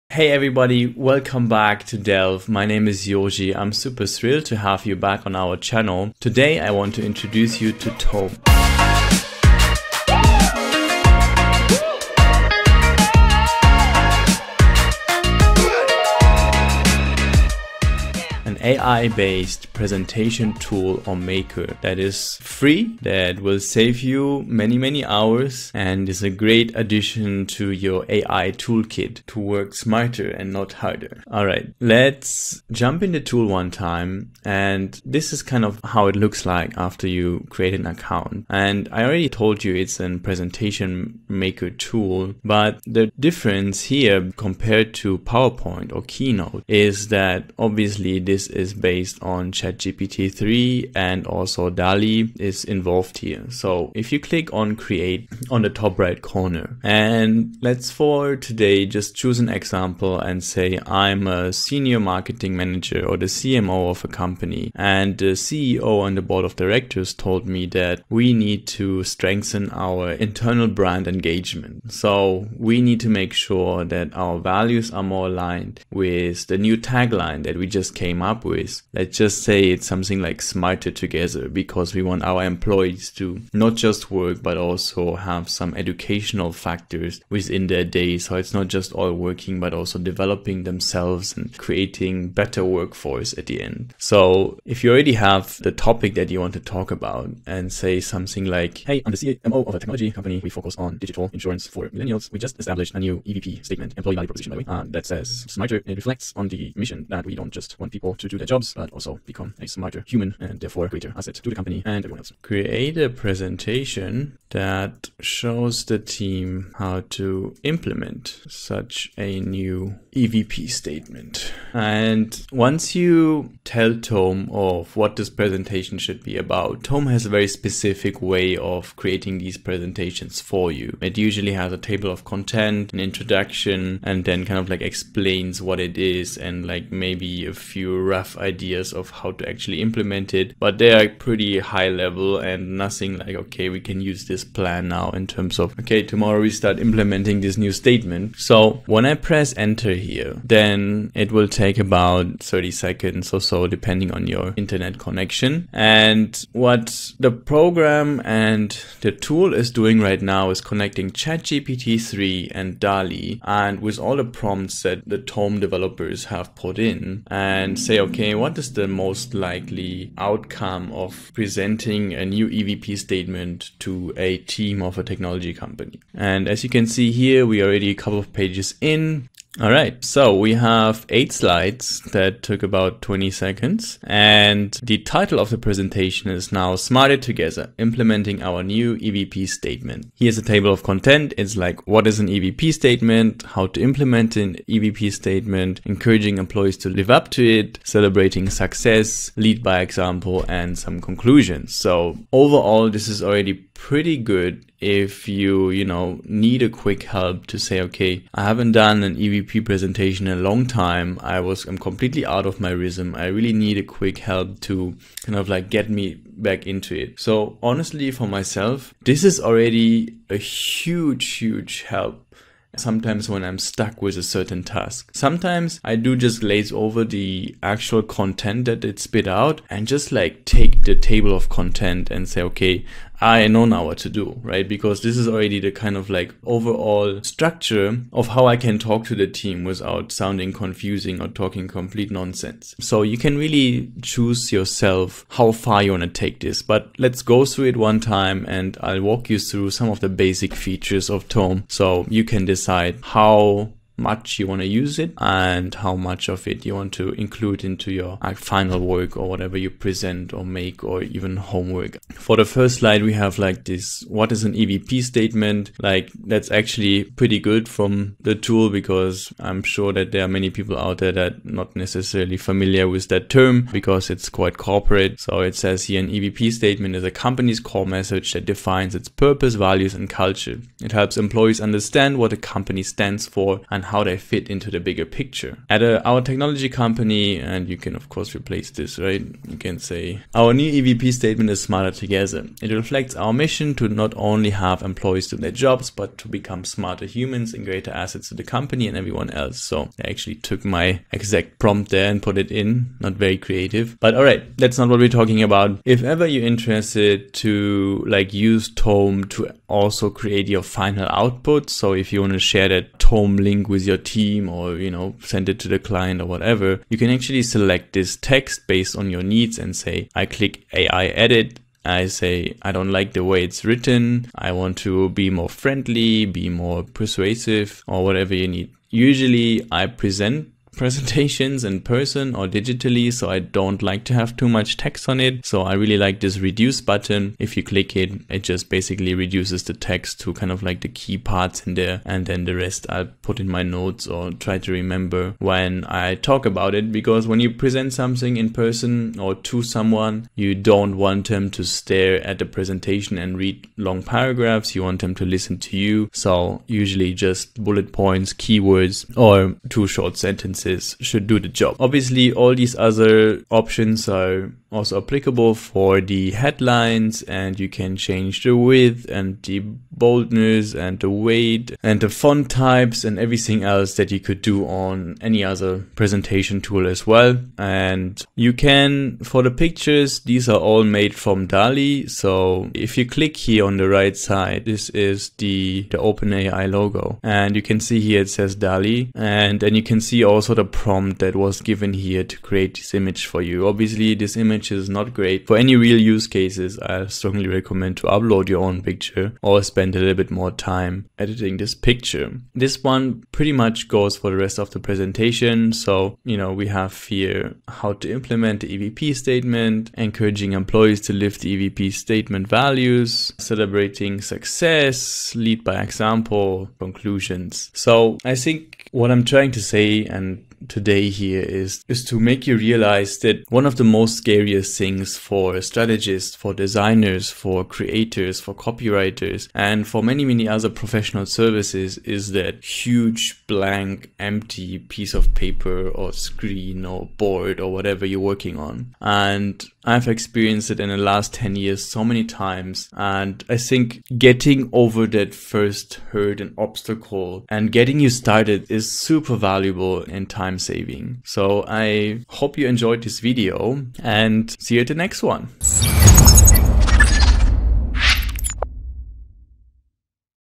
Tome - The AI Presentation Maker (Ai Tools)
Welcome to Dellve, where we explore the latest technology and tools for businesses and creators. In this video, Yohji introduces you to Tome, an AI-powered presentation maker tool based on ChatGPT. Discover how this powerful tool can help you create engaging and effective presentations in no time and help you summarize and collect your thoughts.

Level up your presentation and communication game with us!